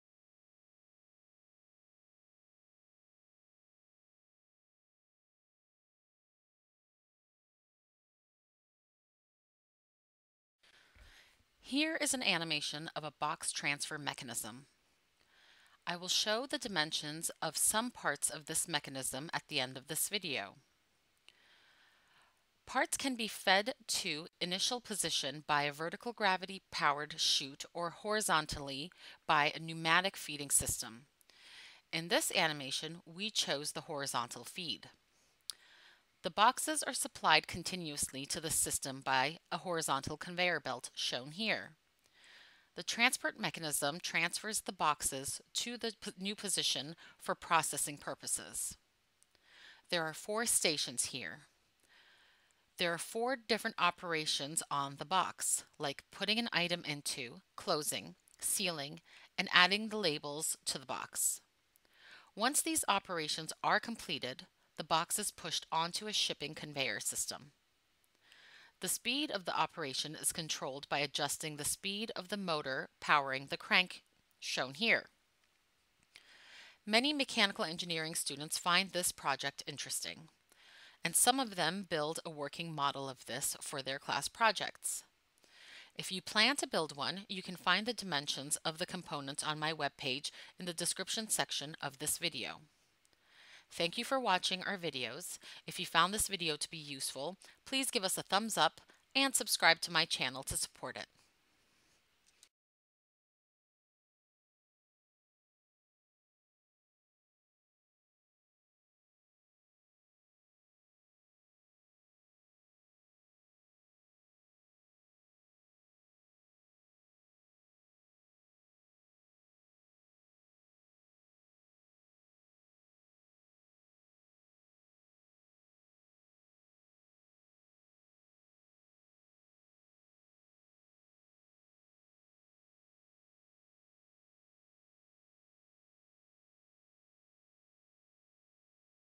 box transport mechanism with human voice
Here is the new version of box transport mechanism recorded with human voice. Also dimension of three important component included in this video.